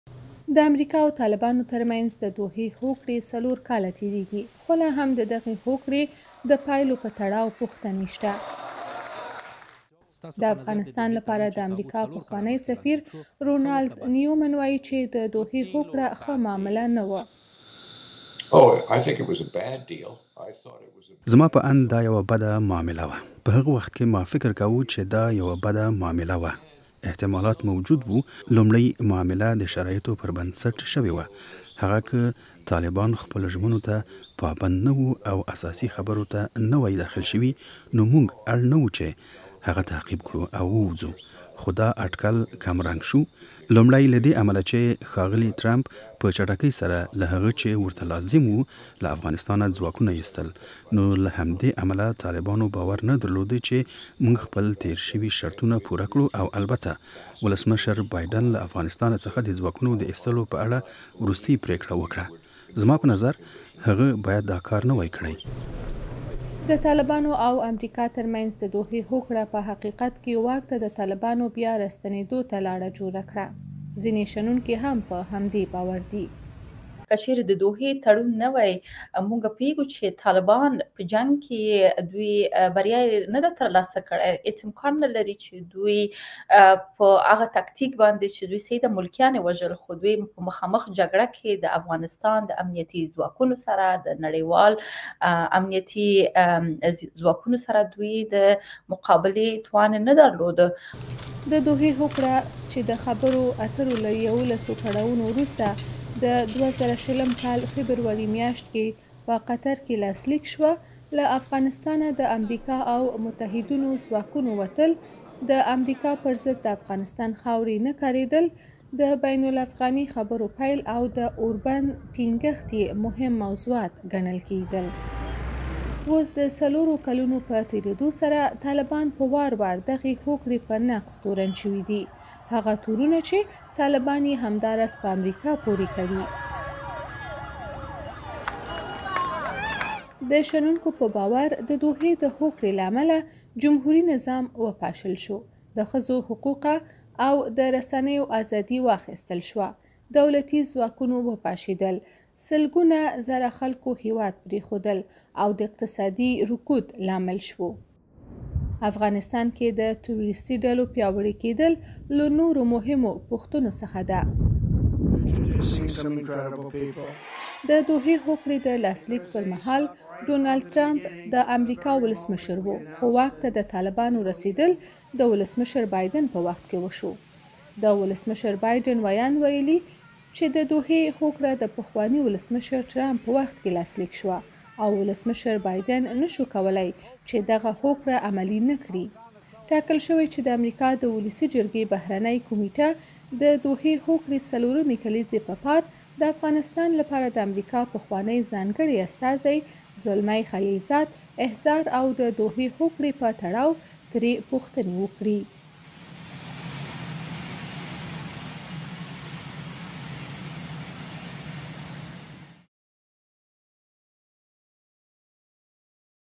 د دوحې تړون څلورمه کلیزه؛ د امریکا پخوانی سفیر: دغه تړون یوه ښه معامله نه وه
د امریکا او طالبانو ترمنځ د دوحې جنجالي تړون په څلورمه کلیزه کې لا تر اوسه هم د دې تړون پر څرنګوالي او پایلو په تړاو پوښتنې شته دي.
په افغانستان کې د امریکا پخواني سفیر ازادي راډیو ته وویل، د دوحې تړون یوه "ښه معامله نه وه."